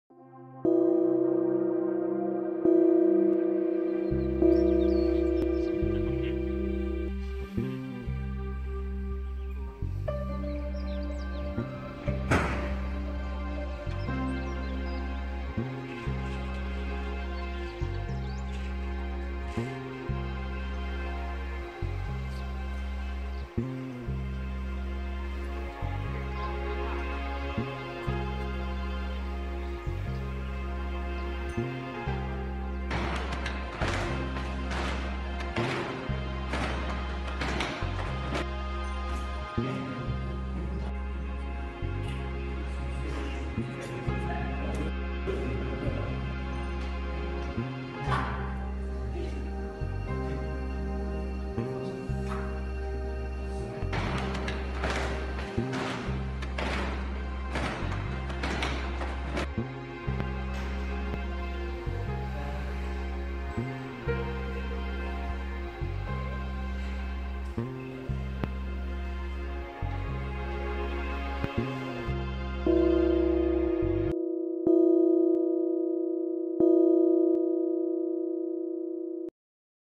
teaching 100 students
- produced in Spring 2017 -
Here i was helping my Master Tang Li Long to teach 100 students in Wudangshan.

Hier habe ich meinem Meister Tang Li Long geholfen 100 Schüler zu Unterrichten.